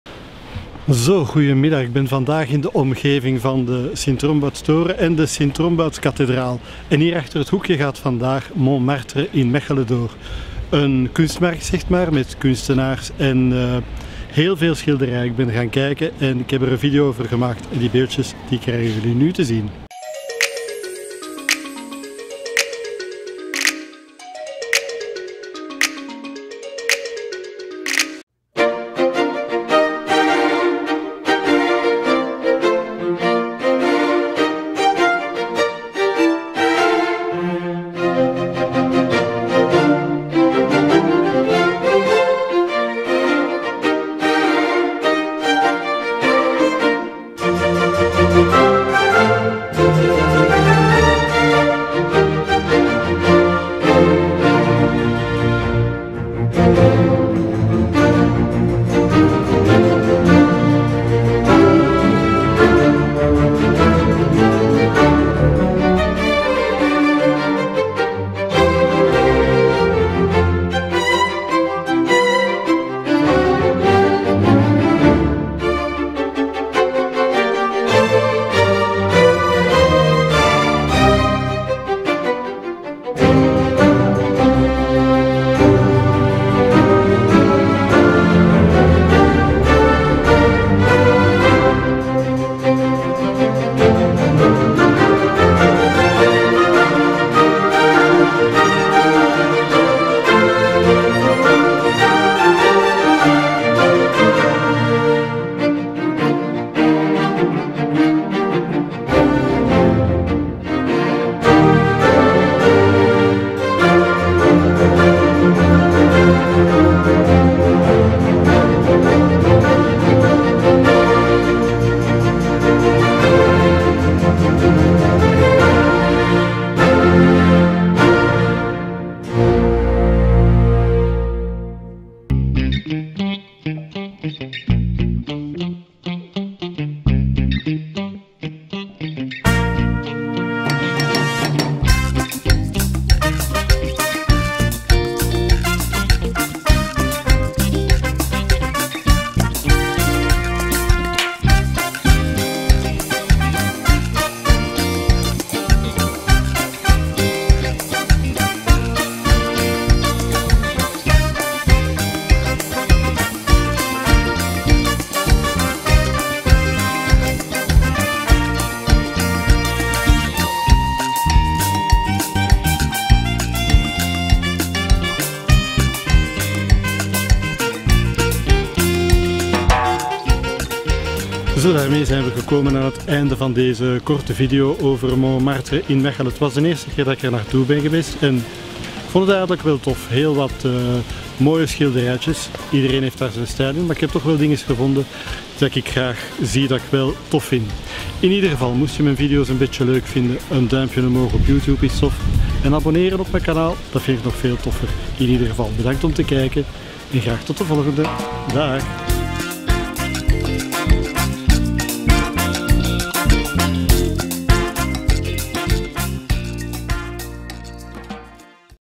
Montmartre in Mechelen Art en Painting
Montmartre  art en painting in Mechelen

Op 1 september 2019 ging het evenement Mormartre in Mechelen door in de schaduw van de Sint Romboutstoren.
Deze kunstmarkt geeft kunstenaars de gelegenheid om hun kunst aan het grote publiek te tonen.
En het publiek is ook met grote aantallen komen kijken. Deze markt gaat dan ook op een unieke locatie door. De omgeving van de Kathedraal is uniek voor dit soort evenement.
De nadruk lag dit jaar blijkbaar toch op de verschillende soorten schilderwerken en geschiedenisboeken.
Op het pleintje kon men genieten van live muziek en een hapje en drankje.
Ik ben er een kijkje gaan nemen en heb er dit kort verslag van  gemaakt.

Veel plezier met deze video 😉

*Alle rechten, exclusief de muziek op deze video, behoren toe aan Michael Grieve
Muziek: Youtube Bibliotheek:

Word ook lid van mijn Facebook groep “Welkom in Mechelen”

Meer weten over Mechelen?